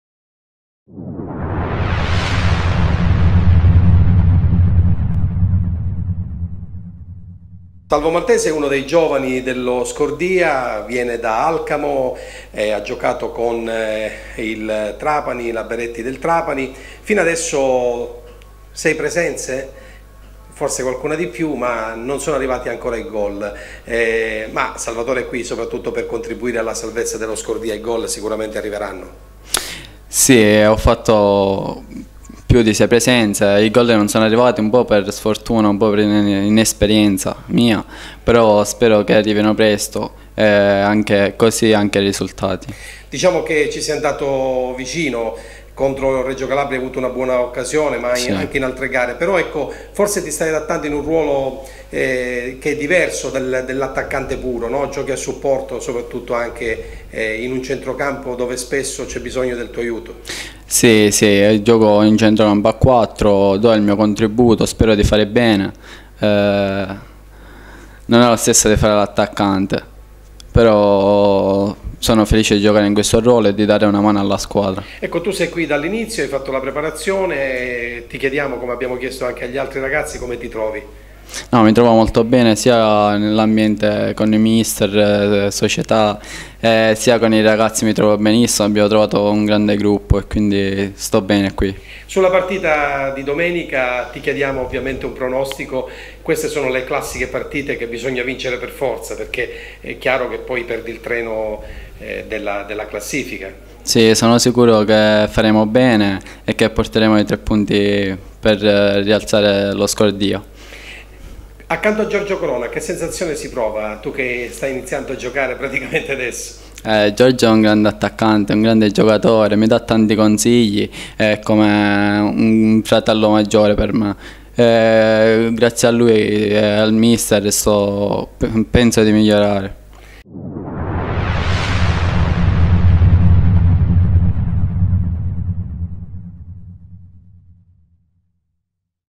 Salvo Maltese
Il giovane attaccante di Alcamo in sala stampa alla vigilia di Scordia - Gelbison